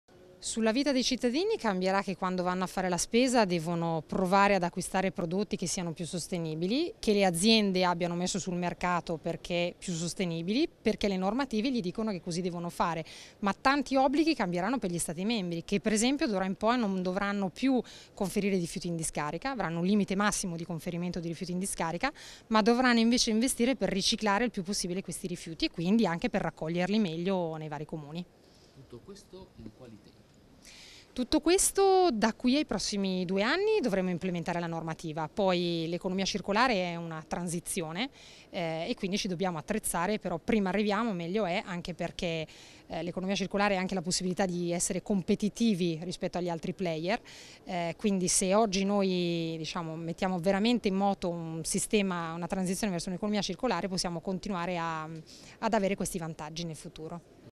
Bonafe' (Pd): "Nuove leggi su economia circolare in vigore entro due anni"
(Agenzia Vista) Strasburgo, 17 aprile 2018
L'eurodeputata del Pd Simona Bonafè a Strasburgo